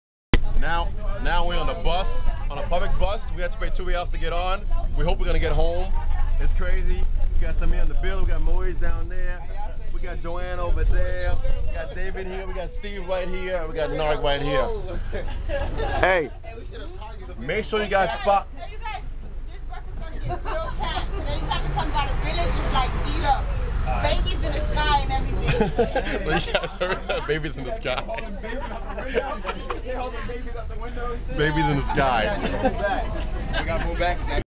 pan am games team haiti riding a public bus with the locals!
Well, after the RIo bus dropped us at the mall we had to catch a cab to get back to the village or try our chance with the locals on the bus! It was cool...